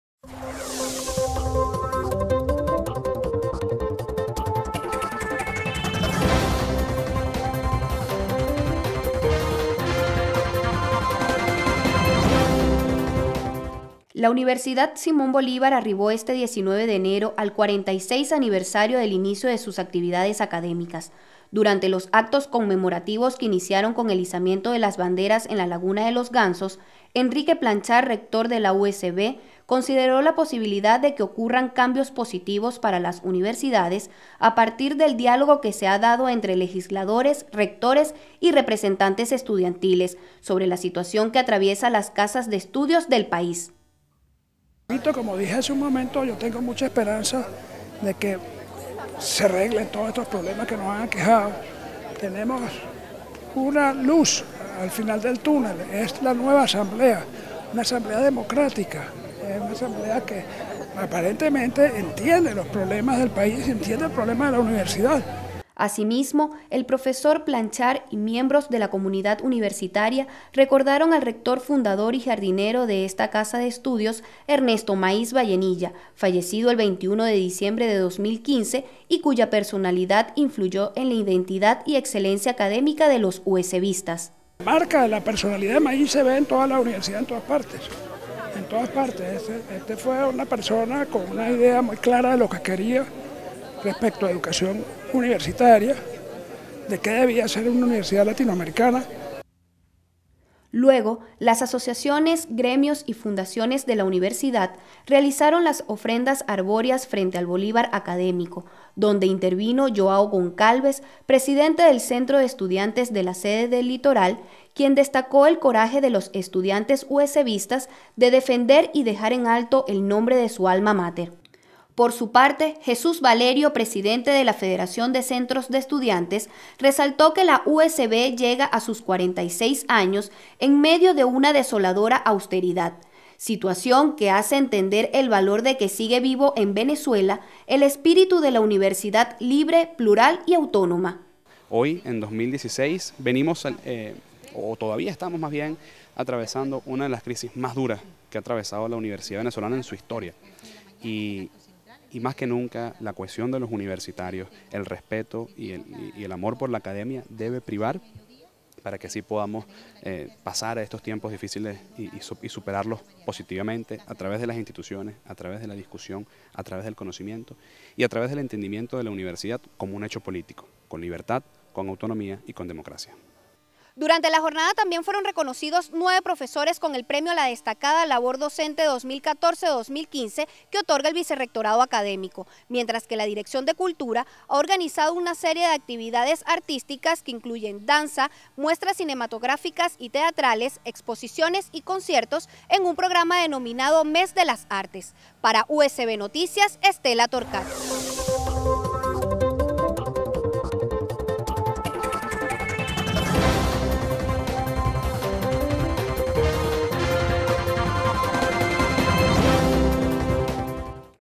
46 Aniversario del inicio de actividades académicas de la USB
La Universidad Simón Bolívar festejó su 46 aniversario de inicio de actividades académicas. En su discurso, el rector Enrique Planchart auguró cambios positivos para las universidades, a partir del diálogo que se ha dado entre los legisladores de la nueva Asamblea Nacional -electos el 6 de diciembre- y el sector universitario, lo que ha permitido dar a conocer la crítica situación que  atraviesan las casas de estudios.

Las actividades conmemorativas del 19 de enero fueron en honor al rector fundador de esta casa de estudios, Ernesto Mayz Vallenilla, fallecido el 21 de diciembre de 2015.